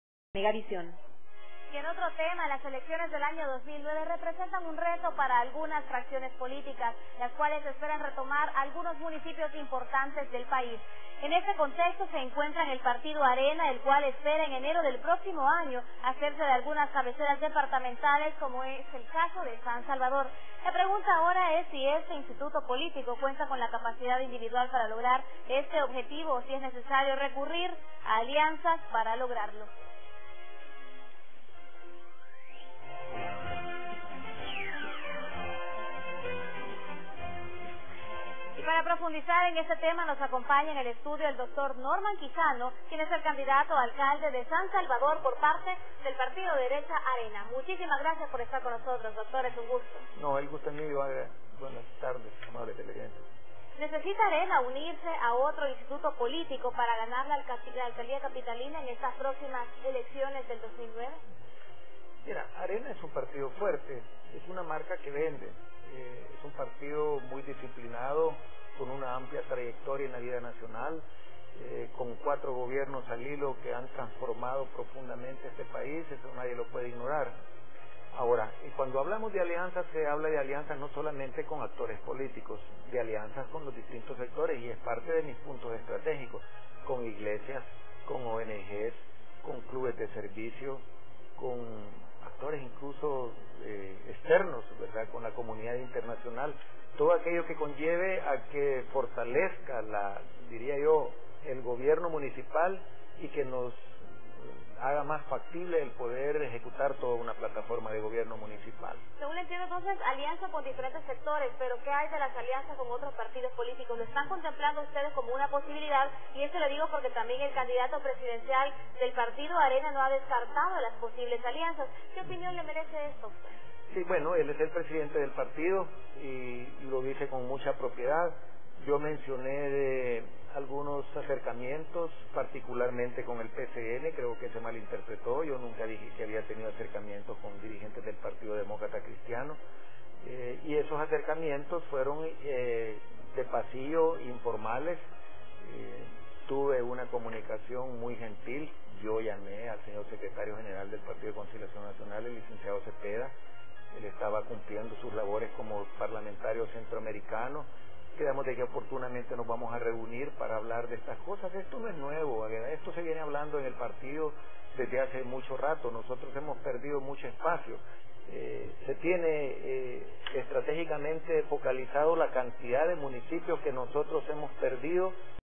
Norman Quijano Alcalde - Entrevista Megavision
Entrevista realizada por el Noticiero Megavision